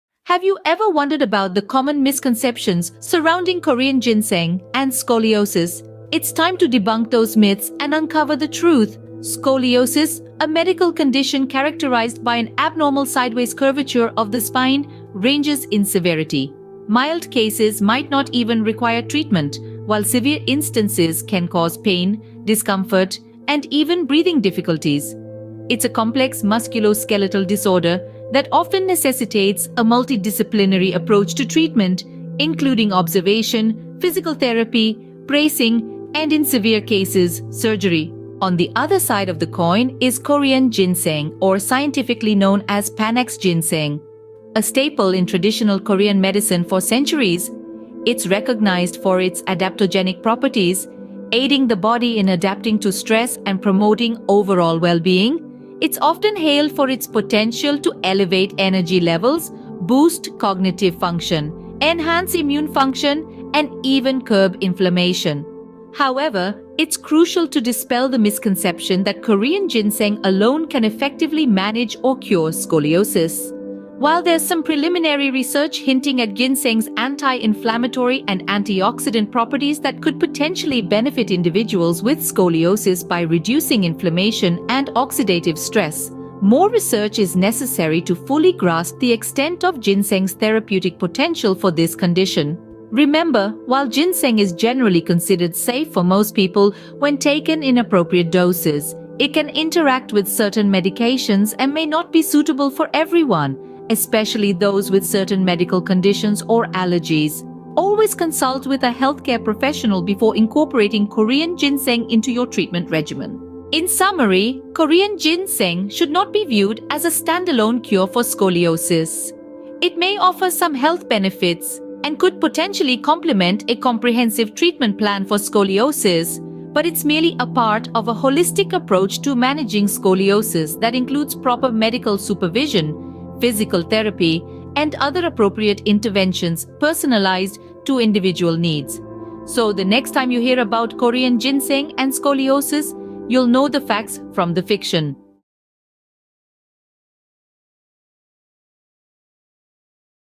Korean Ginseng and Scoliosis A Balanced Perspective
While Korean ginseng is renowned for its potential health benefits, it's important to address its relationship with scoliosis with accuracy and caution. Scoliosis is a medical condition characterized by an abnormal sideways curvature of the spine. It can vary in severity, from mild cases that may not require treatment to severe cases that can cause pain, discomfort, and even breathing difficulties.

Korean ginseng, scientifically known as Panax ginseng, has been used for centuries in traditional Korean medicine and is celebrated for its adaptogenic properties, meaning it may help the body adapt to stress and promote overall well-being. It's often touted for its potential to boost energy levels, improve cognitive function, enhance immune function, and even reduce inflammation.

However, when it comes to treating scoliosis specifically, there is limited scientific evidence to support the claim that Korean ginseng alone can effectively manage or cure the condition. Scoliosis is a complex musculoskeletal disorder that often requires a multidisciplinary approach to treatment, which may include observation, physical therapy, bracing, and in severe cases, surgery.

That being said, there is some preliminary research suggesting that ginseng may have anti-inflammatory and antioxidant properties that could potentially benefit individuals with scoliosis by reducing inflammation and oxidative stress in the body. These effects may help alleviate some of the discomfort associated with scoliosis and support overall health, but more research is needed to fully understand the extent of ginseng's therapeutic potential for this condition.

It's also important to note that while ginseng is generally considered safe for most people when taken in appropriate doses, it can interact with certain medications and may not be suitable for everyone, particularly those with certain medical conditions or allergies. As with any herbal supplement, it's crucial to consult with a healthcare professional before incorporating Korean ginseng into your treatment regimen, especially if you have scoliosis or any other medical condition.

In conclusion, while Korean ginseng may offer some health benefits and could potentially complement a comprehensive treatment plan for scoliosis, it should not be viewed as a standalone cure for the condition. Rather, it should be considered as part of a holistic approach to managing scoliosis that includes proper medical supervision, physical therapy, and other appropriate interventions tailored to individual needs.